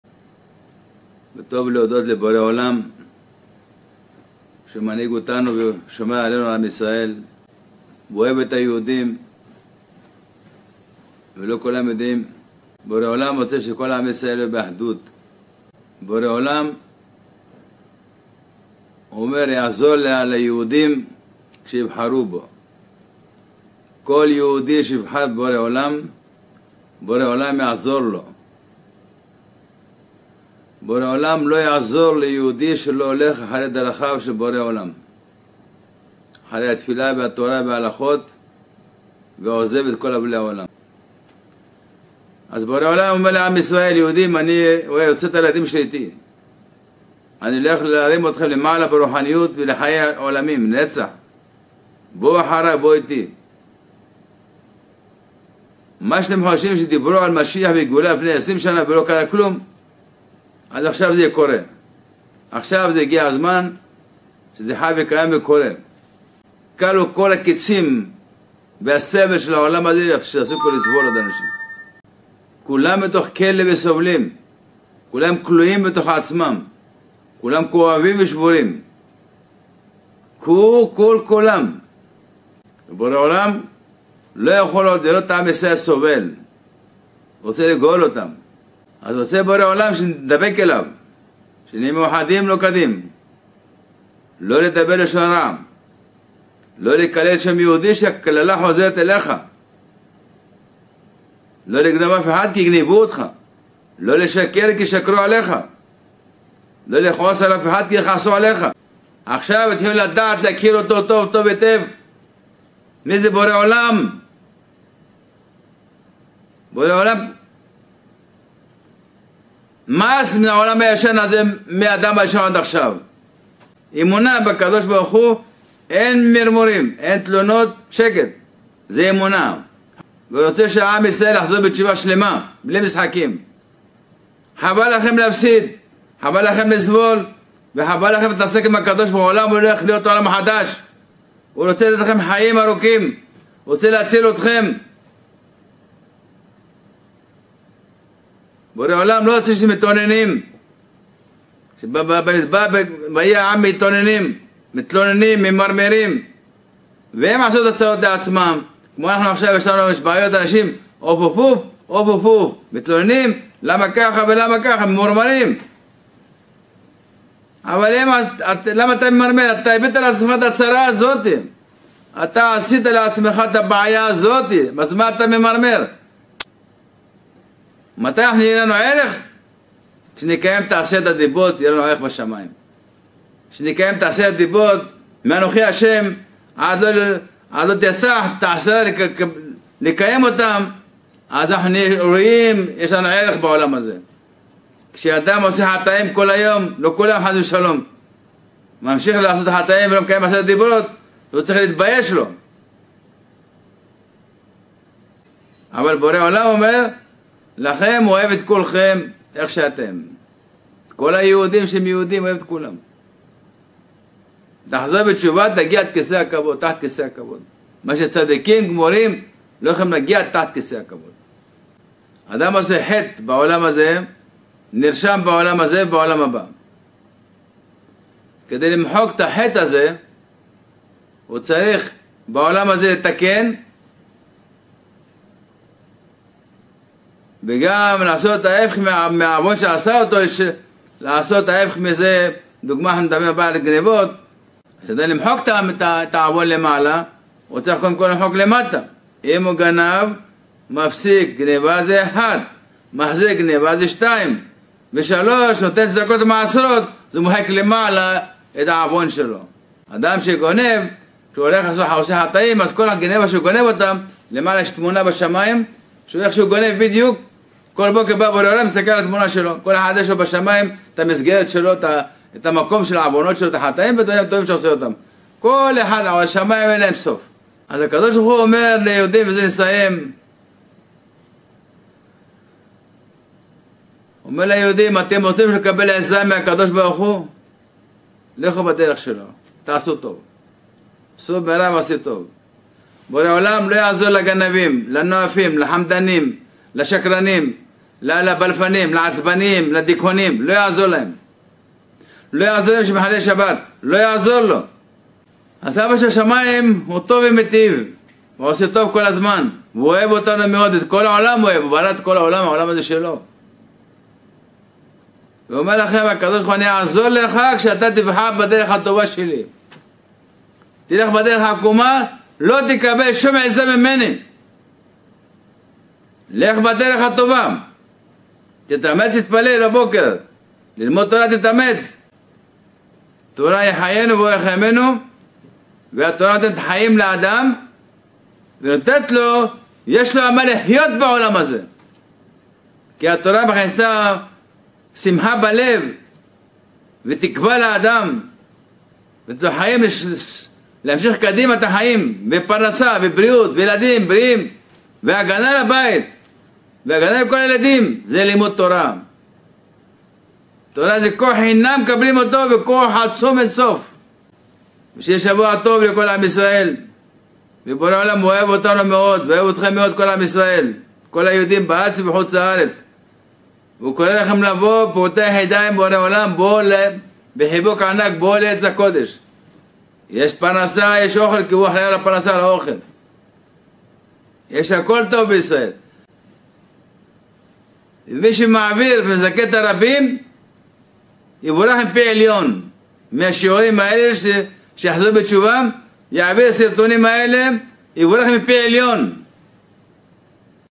משיח ה' הרב ניר בן ארצי אומר כלו כל הקיצים של העולם הזה ובורא עולם מאס בעולם הישן!
משיח ה' הרב ניר בן ארצי אומר הקב"ה אוהב את כל היהודים ורוצה שכולם יהיו באחדות. כל יהודי שיבחר בבורא עולם הבורא יעזור לו. הבורא לא יעזור ליהודי שלא הולך אחר דרכיו.
כל כולם סובלים כלו כל הקיצים של העולם הזה, בורא עולם מאס בעולם הישן. אז רוצה שידבקו אליו, שישמרו את עשרת הדיברות, שיחזרו בתשובה שלמה. הולך להיות עולם חדש חבל להפסיד ולסבול! לא להתמרמר! תתקנו את החטאים ותעשו במקום מעשים טובים כך תמחקו את הצילום של החטאים בשמים. סור מרע ועשה טוב! ללמוד תורה שמכניסה שמחה בלב ותקווה וחיים לאדם להמשיך קדימה.
מי שיעביר את הסרטונים האלה שיחזרו בתשובה יבורך מפי עליון!!!